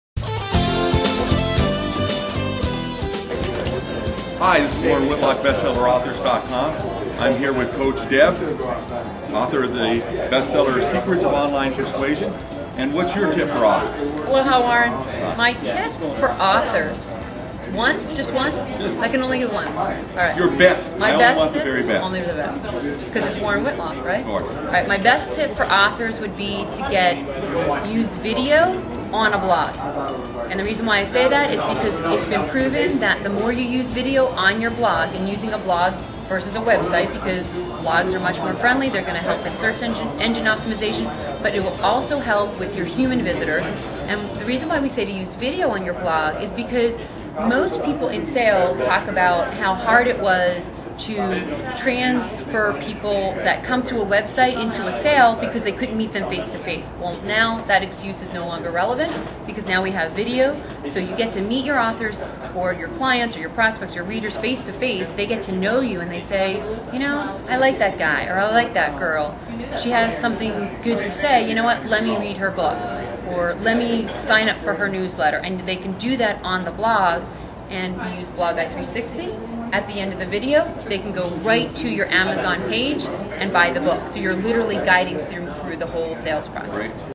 Authors Use Video On Their Blogs To Sell Books
Book marketing and promotion works best when your audience sees you. Attract readers, sell books and be a best seller